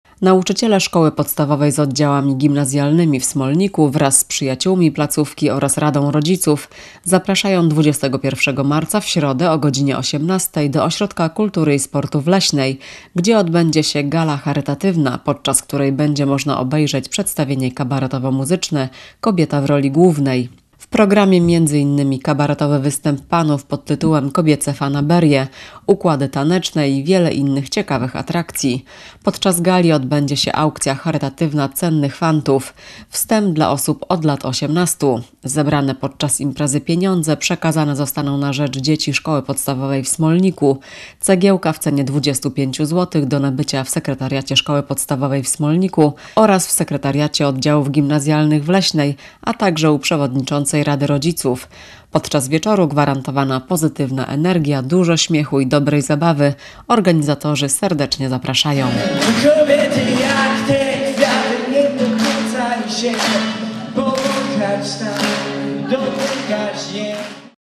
Kobiece fanaberie i nie tylko
21 marca w Ośrodku Kultury i Sportu w Leśnej odbędzie się Gala Charytatywna, na której będzie można zobaczyć między innymi kabaretowe przedstawienie panów z Leśnej pod tytułem „Kobiece fanaberie”. Wielki zastrzyk dobrej zabawy gwarantowany.